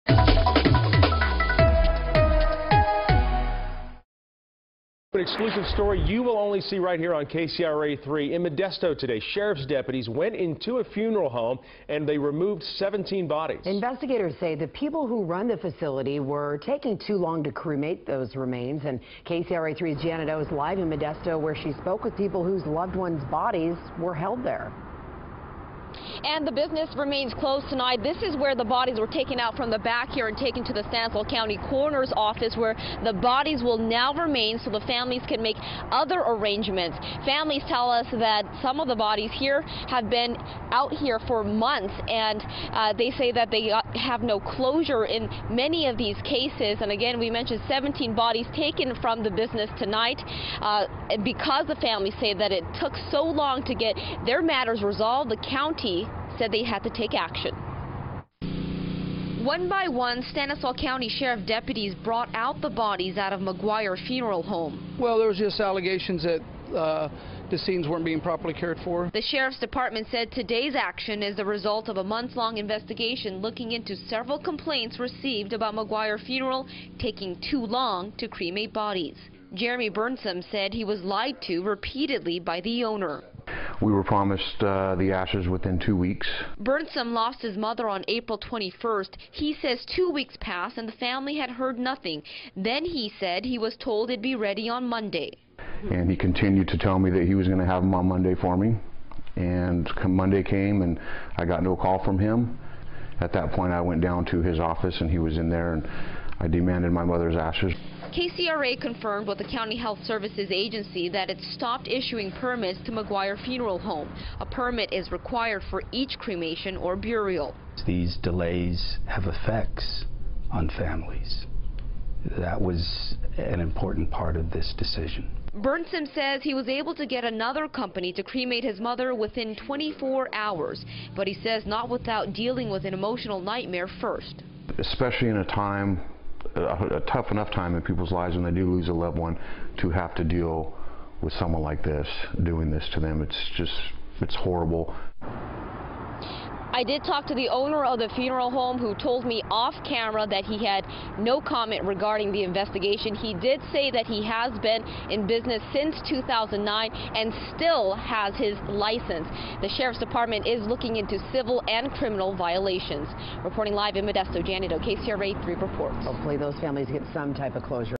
Deputies take 17 bodies from Modesto funeral home
Stanislaus County sheriff's deputies went into a Modesto funeral home on Friday and removed 17 bodies because the facility was taking to long to cremate the remains.